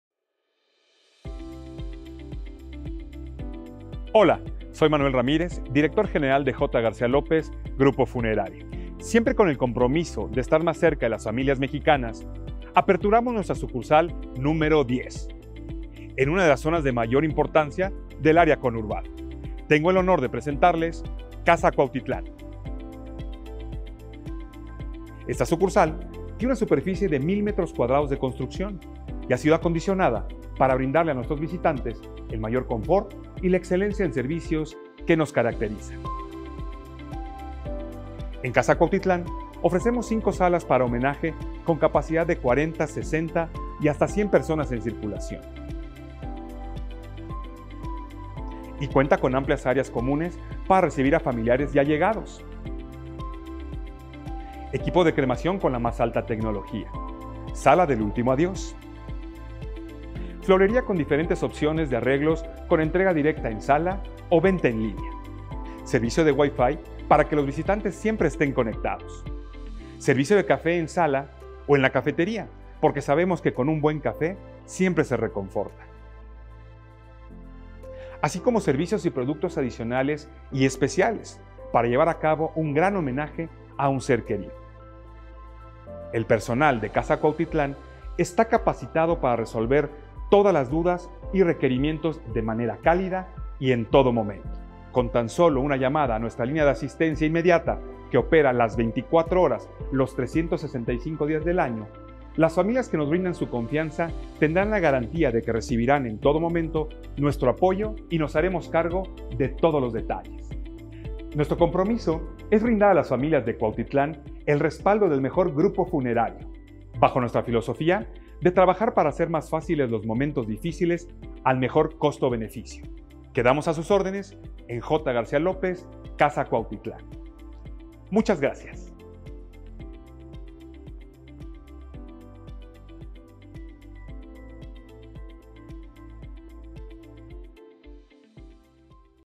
J. García López | Nueva Sucursal Casa Cuautitlán
Tenemos el honor de presentar nuestra sucursal número 10, Casa Cuautitlán cuyas instalaciones brindan el mayor confort y la excelencia en el servicio que distingue a J. García López.